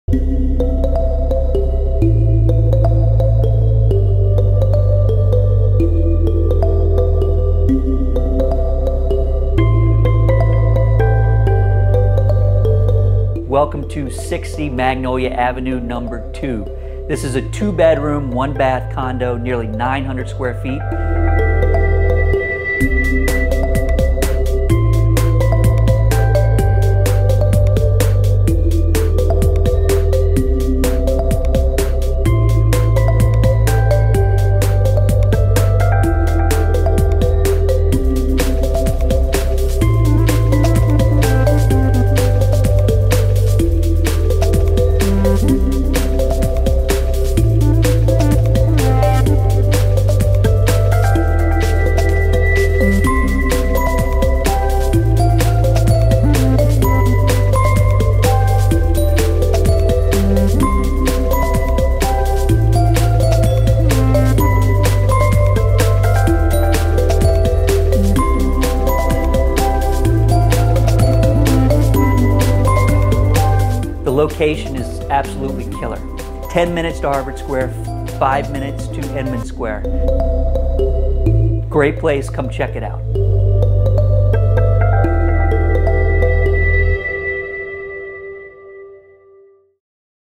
60 Magnolia #2
Recently renovated to the studs 2bd/1ba condo, located minutes away from both Inman and Harvard Squares.

This well-designed home features gleaming hardwood floors throughout, recessed lighting, a spectacular granite/stainless kitchen with ample storage, beautifully tiled bathroom, gas fireplace, in-unit laundry, central air/heat, private basement storage and an over-sized covered deck off the spacious living room.

Steps from the Cambridge Public Library as well as the popular Wine & Cheese Cask, Bergamot, Dali, Kirkland Tap & Trotter, Savenor's Market and the Biscuit all on the same corner! A brand new Whole Foods store is just down the road. High owner occupancy, low monthly condo fees and very strong condo cash reserves make this a very wise investment.

Showings start at 2 open houses: Saturday and Sunday 12:30-2pm. Offers will be reviewed on Tuesday 8/12 at 5pm.